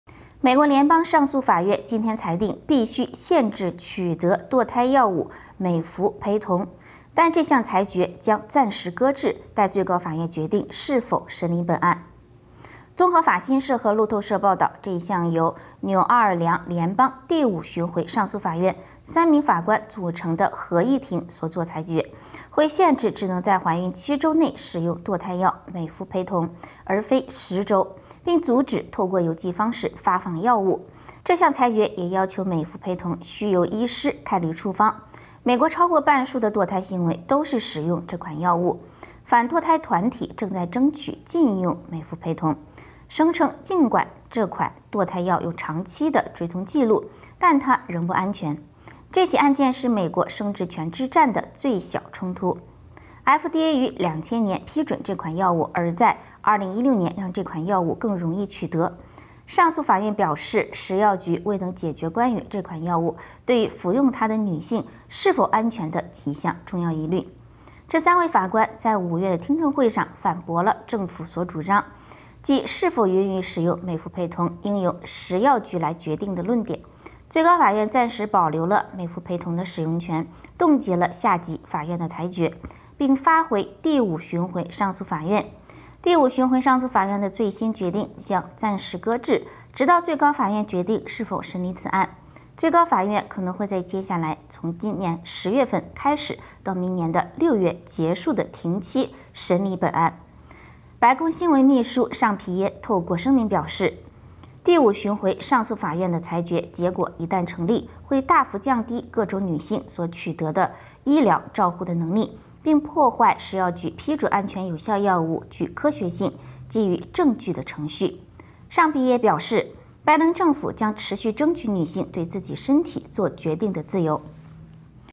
美國聯邦上訴法院今天裁定，必須限製取得墮胎藥物「美服培酮」（mifepristone），但這項裁決將暫時擱置，待最高法院決定是否審理本案
這項由紐奧良聯邦第五巡迴上訴法院3名法官組成的合議庭所做裁決，會限製只能在懷孕7週內使用墮胎藥美服培酮，而非10週，並阻止透過郵寄方式發放藥物。這項裁決也要求美服培酮須由醫師開立處方